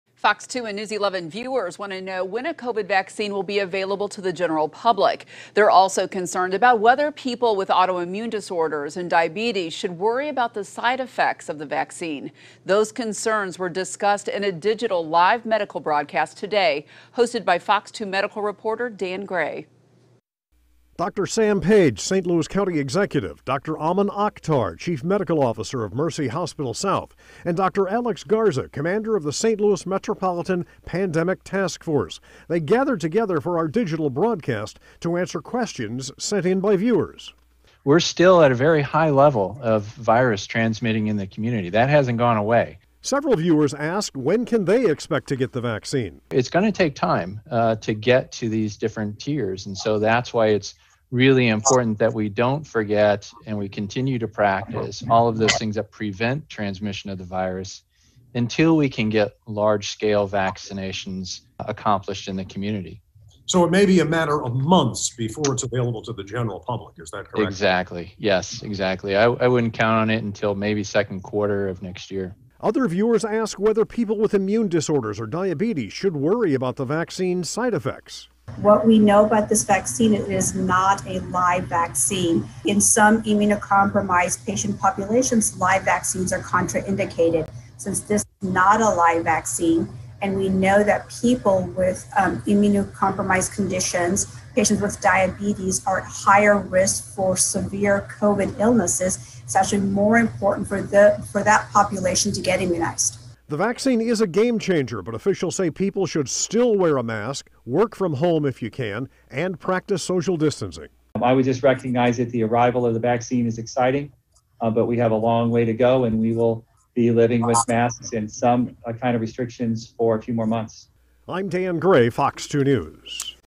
Medical experts take FOX 2/News 11 viewer questions about COVID, vaccine
Our viewers want to know when a COVID vaccine will be available to the general public. They're also concerned and what whether people with autoimmune disorders and diabetes should worry about the side effects of the vaccine.